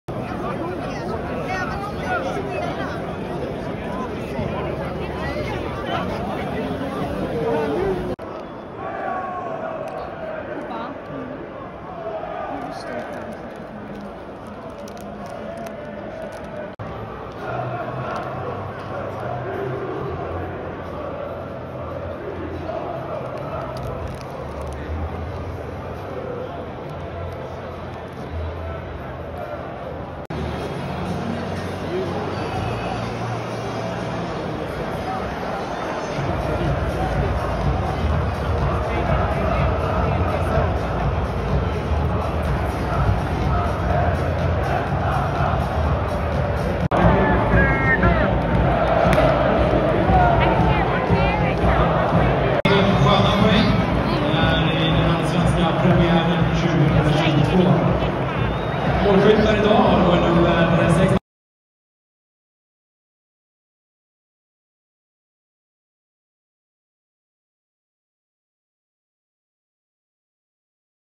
Allsvenskan Hammarby 2-1 Helsingborg match vlogg
Hammarby vann hemmapremiären mot Helsingborg med 2-1.